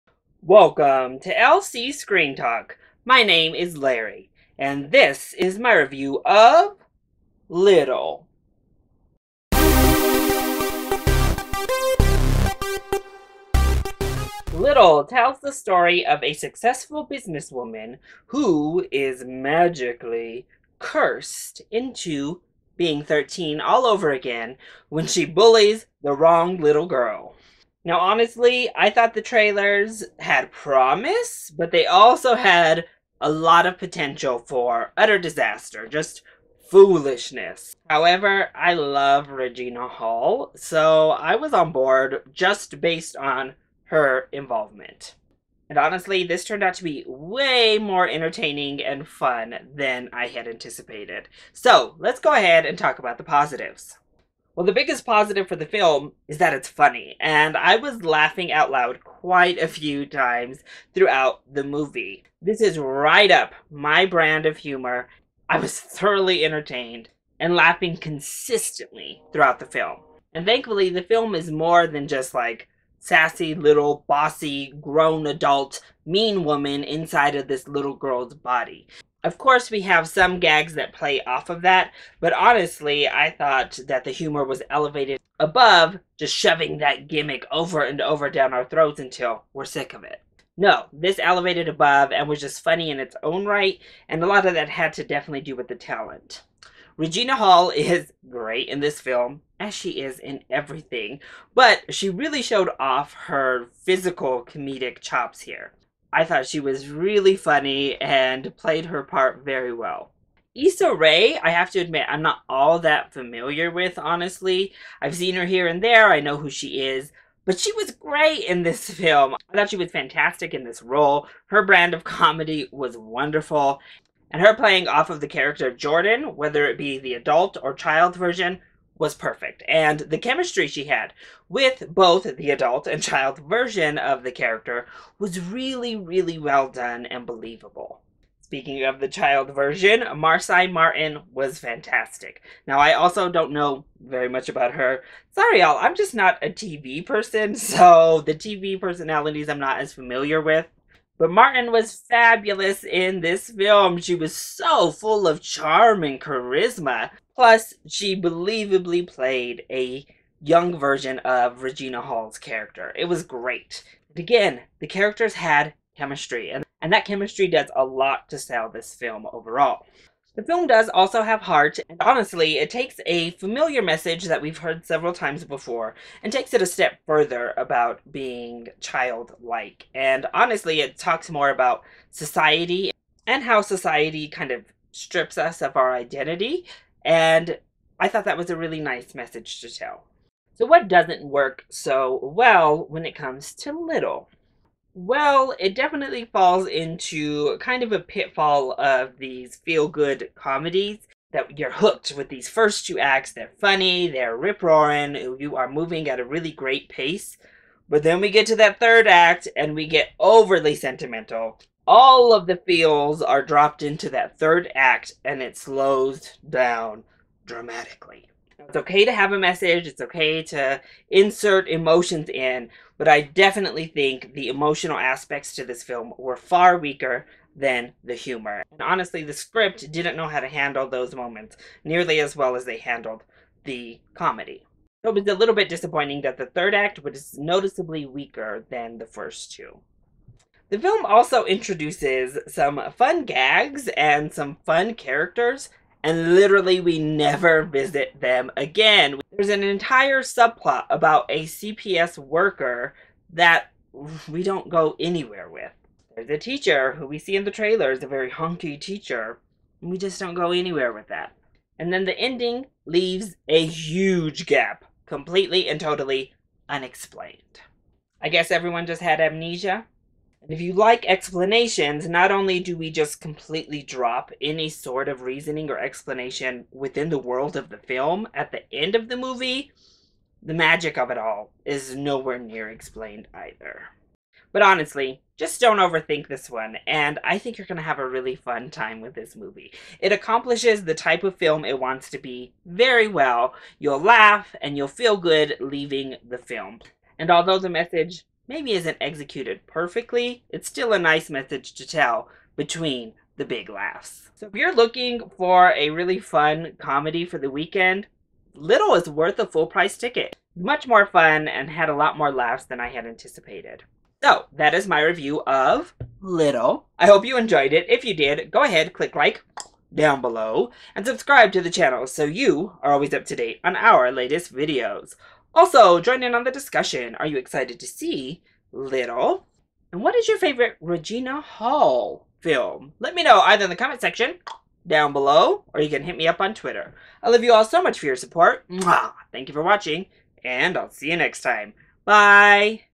Little (2019) Movie Review
LC Screen Talk Larry's review of Little! Does this little comedy produce some big laughs or is the ambition too small to bother?